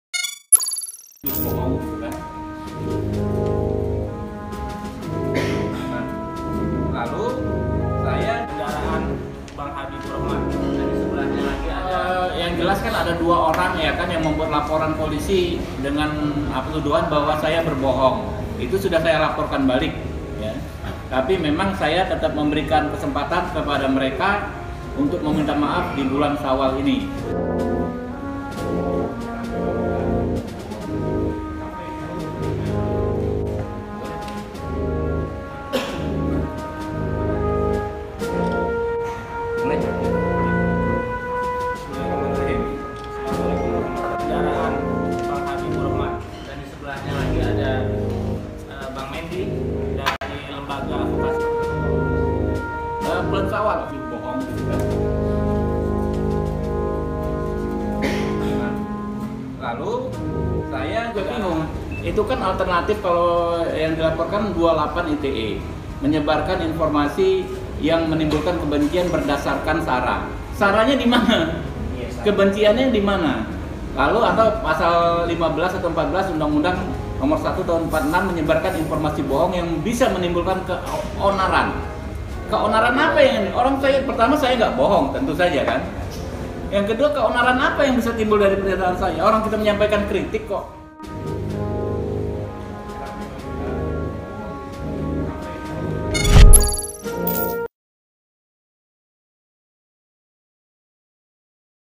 Habiburokhman Beri Kesempatan Pelapor 'Mudik Neraka' Minta Maaf
Ketua Bidang Advokasi DPP Partai Gerindra Habiburokhman memberikan kesempatan kepada pihak yang melaporkannya terkait 'mudik neraka' untuk meminta maaf. Sampai saat ini, sudah ada dua pihak yang melaporkan Habiburokhman ke polisi.